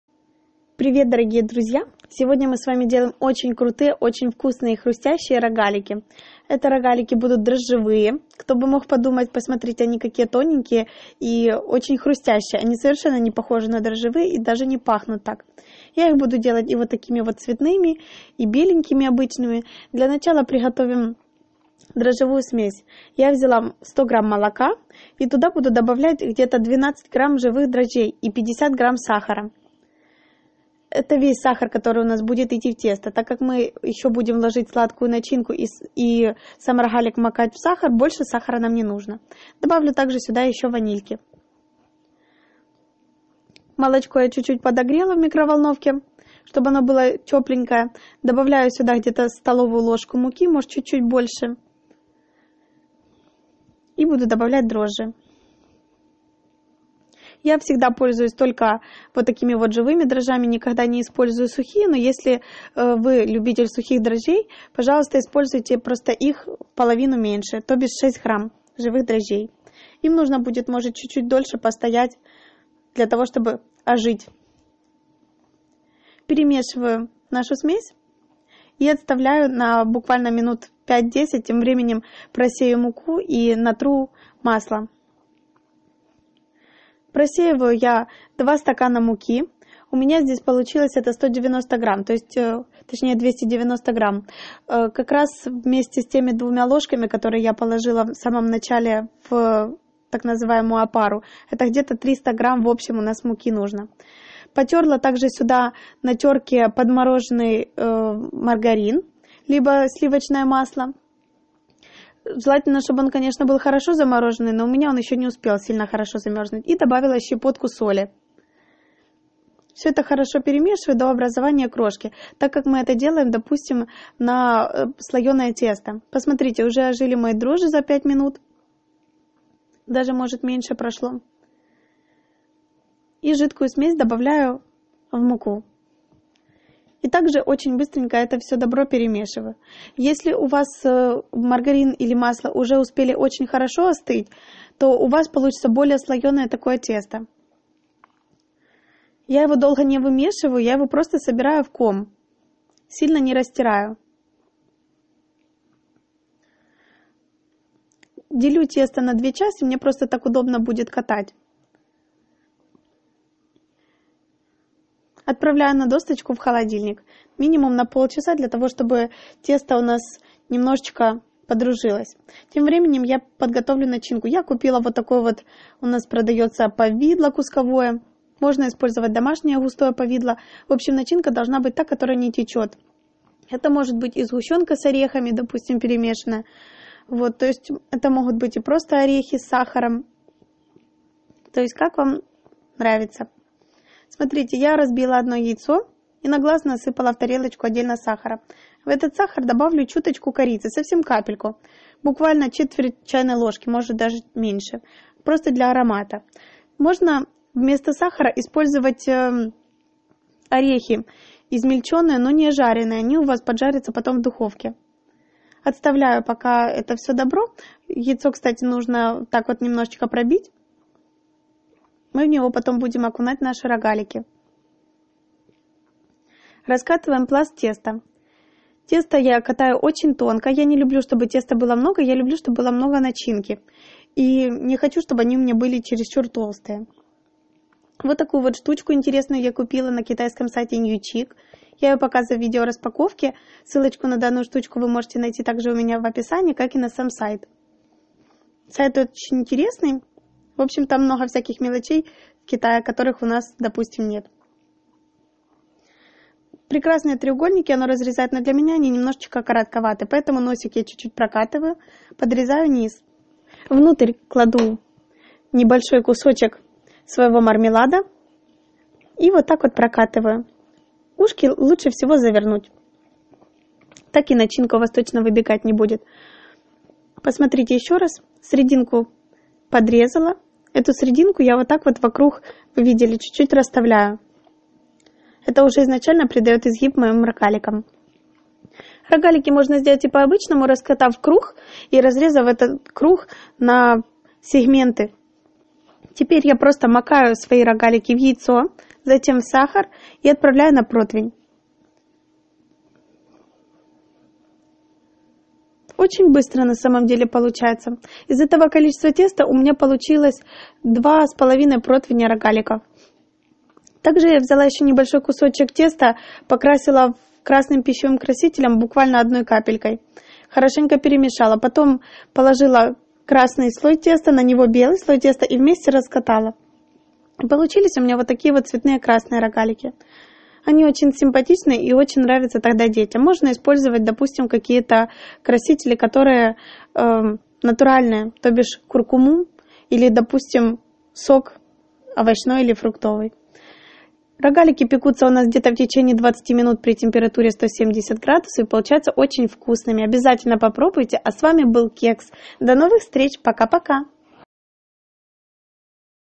Хрустящие слоено-дрожжевые рогалики.Не черствеют!NEWCHIC
Сайт для покупок из китая
обзор последних покупок из китая

Дрожжи 12г прес.(6 сухих)
молоко 100г
сахар 50г
мука 2ст.+2ст.л.
масло или маргарин- 125г
ванилька,корица
повидло или сгущенка